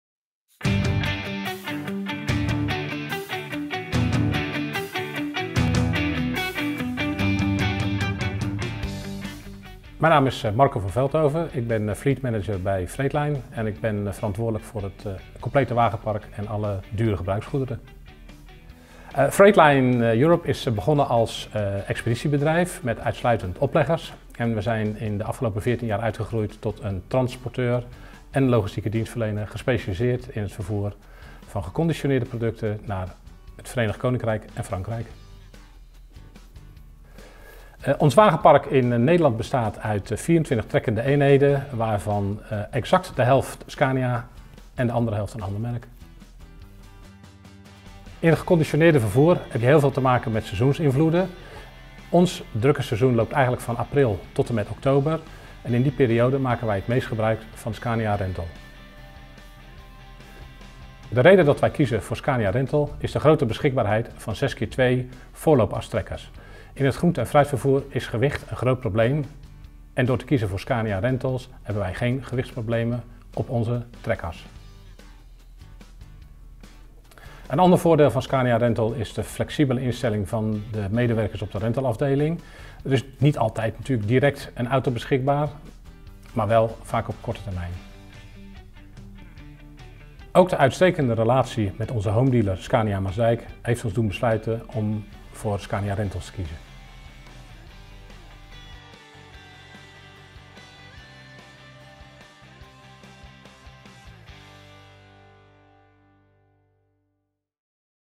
Freight Line Europe “Scania Rental is goed in staat om onze flexibele schil op te kunnen vangen”
In de hectiek van voor de Brexit is het eind 2020 een komen en gaan van vrachtwagens bij Freight Line Europe. Ook al is het bedrijf goed voorbereid op de Brexit, de Britten slaan nu flink in. Alle groenten- en fruitmagazijnen in het Verenigd Koninkrijk moeten worden gevuld. Het wagenpark van 24 wagens en de vaste charters zijn niet genoeg voor deze drukte, dus huurt Freight Line Europe graag wat extra wagens in bij Scania Rental.